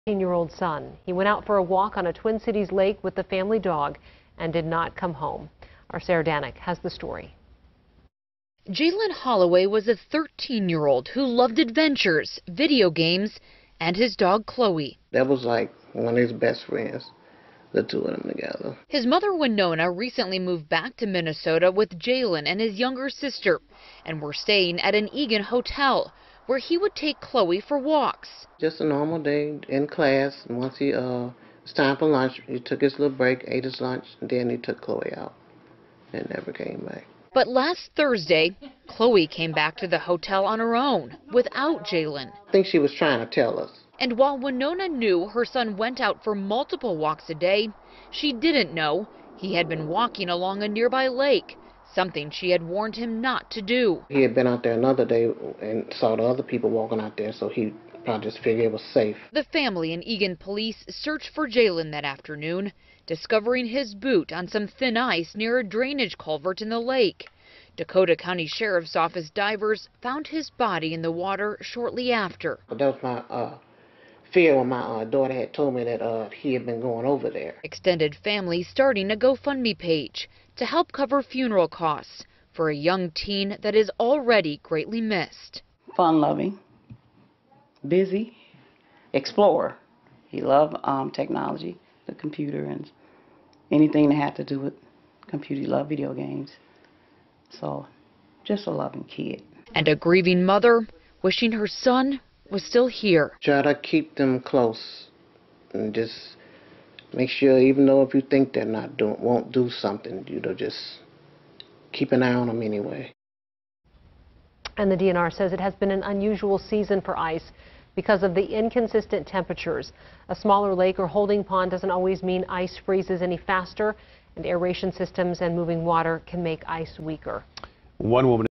Family mourns loss of teen who fell through ice on Eagan lake | FOX 9 KMSP
Family members are mourning the loss of a teen boy who fell through the ice on a lake in Eagan, Minnesota.